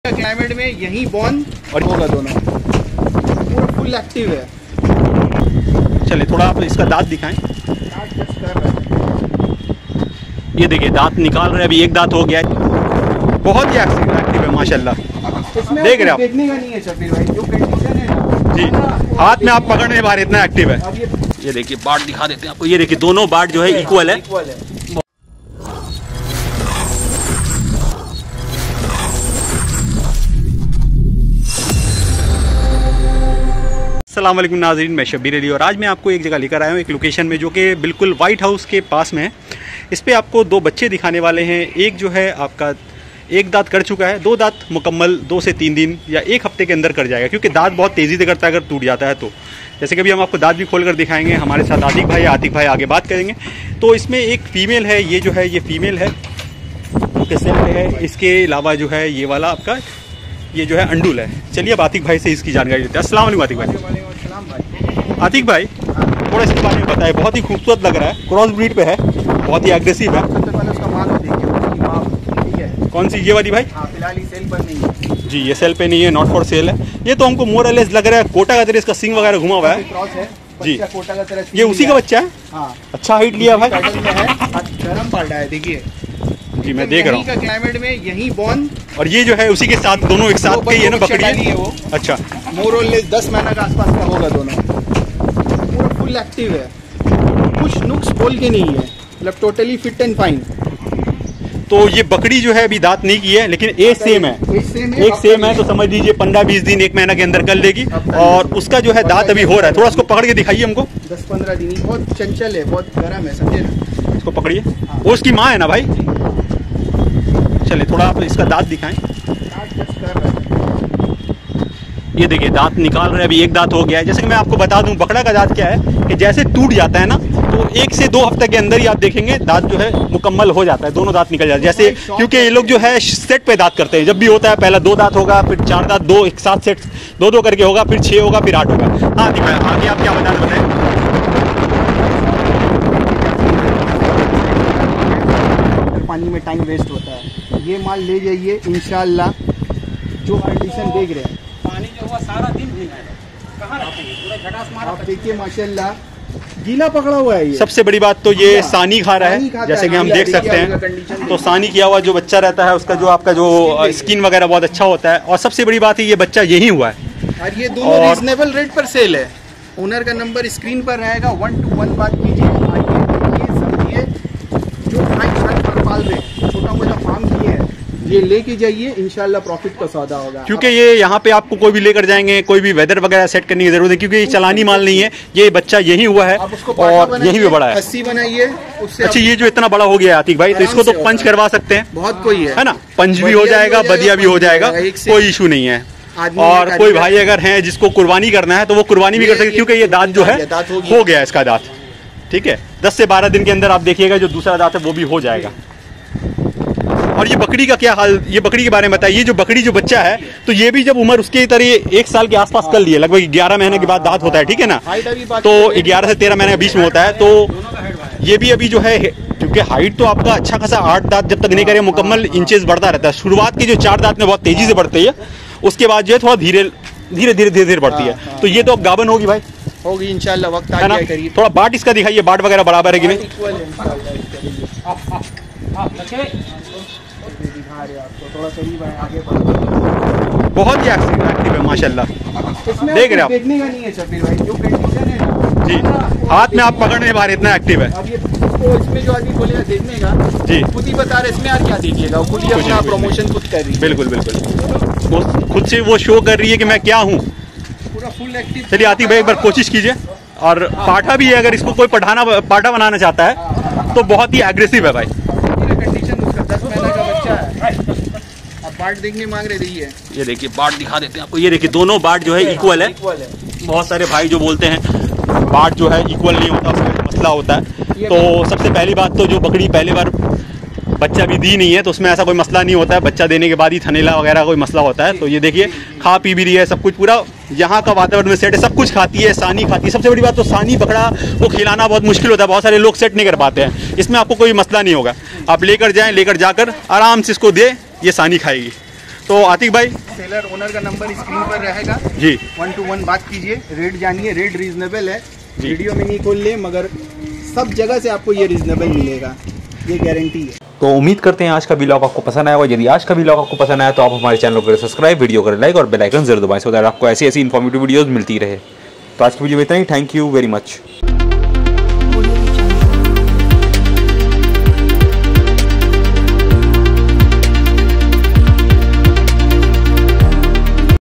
Ghar Pala Bakra & Bakri Jodi For Sale At Reasonable Price | Contact Adil : 6290909496
Ghar Pala Bakra & Bakri Jodi For Sale At Reasonable Price | Location : Zam Zam Briyani, Kolkata

Ghar Pala Goats kid Available For Sale
Climate Set
Fit & Healthy 🐐🐐 Goats

Name= Adil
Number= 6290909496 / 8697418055
Kolkata
Zam zam biryani
Park Circus
For Urgent Sale

Thanks for being a fan!

Please Don't take this negatively.
ThankYou..